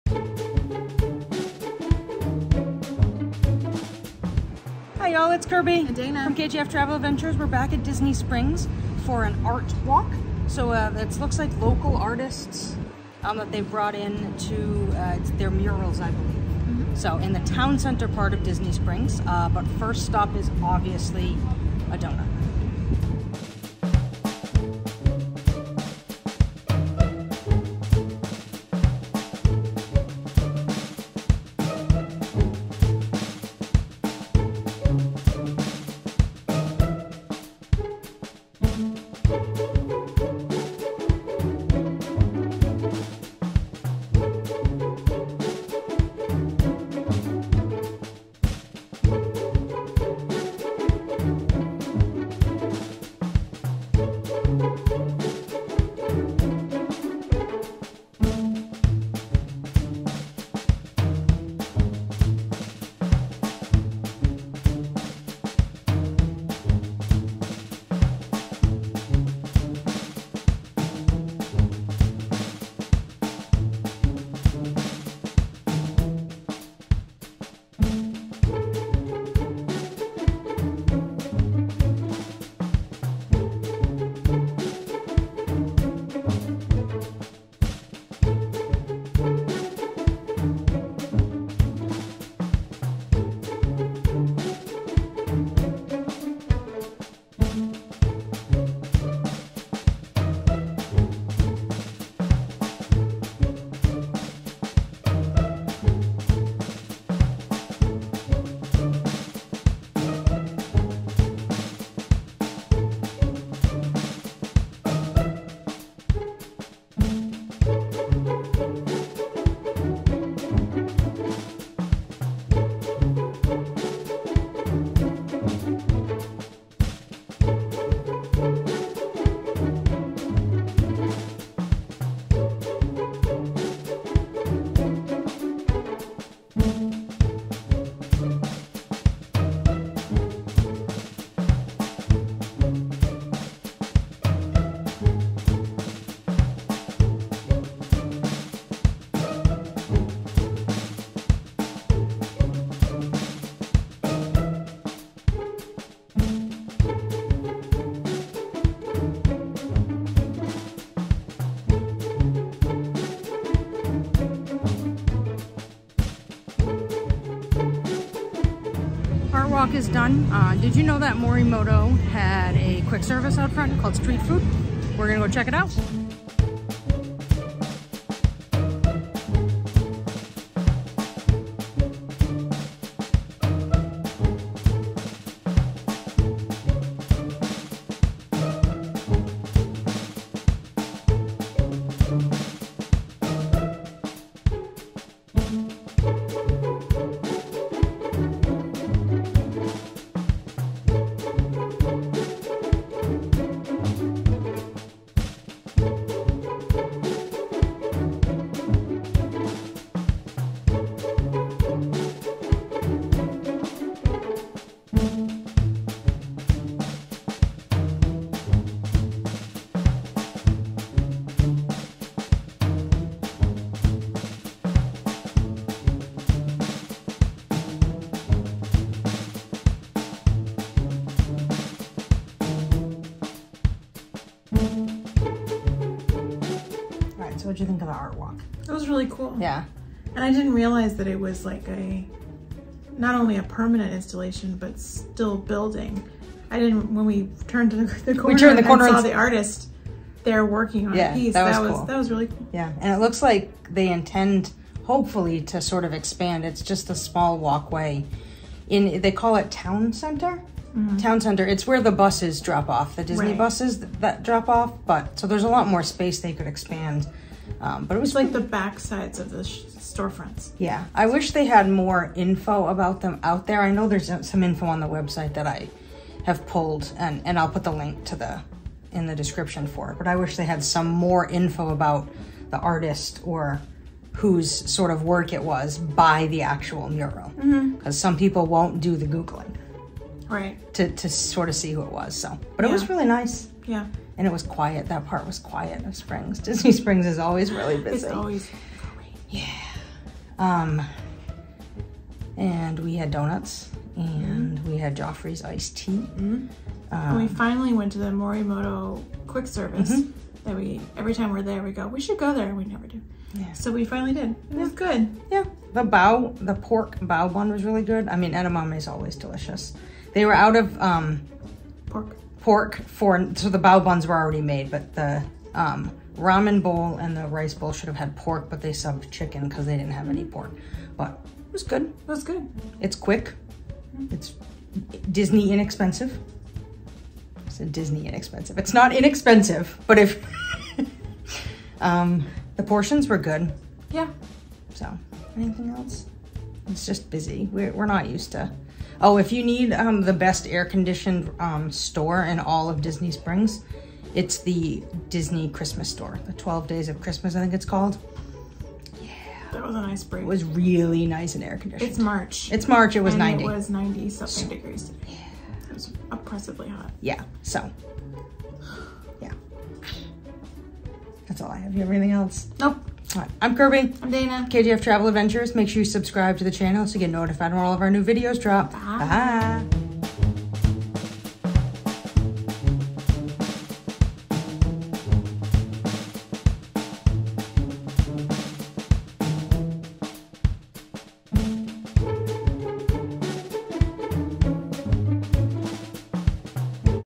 Disney Springs Art Walk: A Canvas of Expression | Everglazed Donuts | Morimoto Street Food
Join KJF-TravelAdventures (now Migratory Nerds) as we head to Disney Springs to check out their newest "attraction" - Art Walk A Canvas of Expression.

If you enjoy our content feel free to buy us a coffee!

We started the trip, as always, with a donut snack from Everglazed Donuts and Cold Brew before exploring the Art Walk.

After a cool drink at Joffrey's we headed to Morimoto Asian to try their quick service option, Street Foods before calling it a night as the crowds continued to thicken.

Techy Stuff!
Video: iPhone 13 Pro
Editing: iMovie

We work for Marriott International however all videos and opinions are our own.